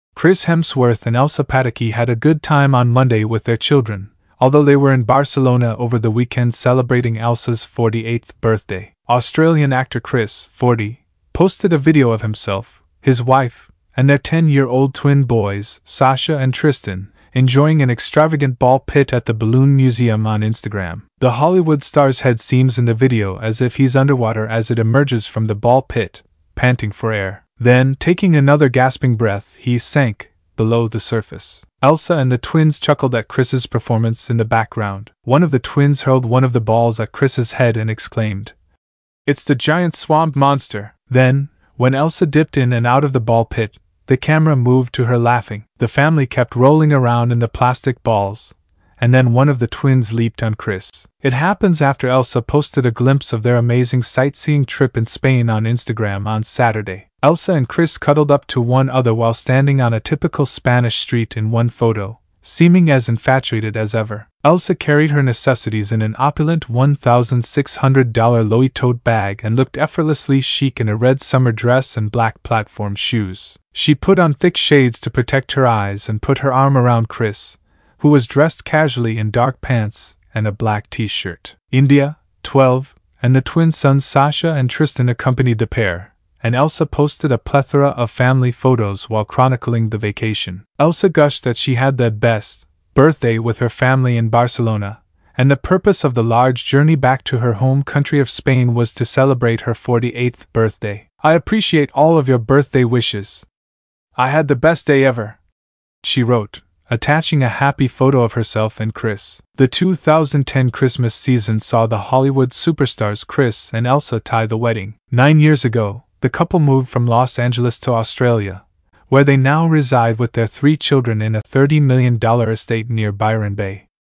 "Hemsworth Family Fun in Ball Pit"
Join Chris Hemsworth and Elsa Pataky as they enjoy playful moments with their kids in an elaborate ball pit at The Balloon Museum in Barcelona. Watch the Hollywood star and his family laugh, play, and make memories during their Spanish getaway. Don't miss out on this heartwarming family fun!